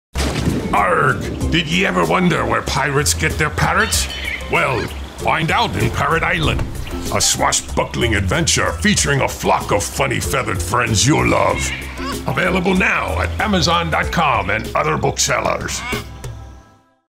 Parrot Island: The Pirate Adventures Book Trailer
So, me hearties, are ye ready for a rollicking adventure on the high seas?  Did ye know where pirates get their parrots? These aren’t ordinary parrots; they come from an exotic island where they’re trained under the watchful eye of Patch at the Semper Fly Academy.  Join the young parrot recruit Gabe, his cracker-snacking best friend Ponce, a ship-smart rat Bilge, a talking barnacle named Barney, and an island of super smart parrots as they battle the crew of pirates lead by the evil first-made Dragut. So come aboard the Rusty Cutlass and set out on an exciting adventure that’s sure to have you shouting arrgh!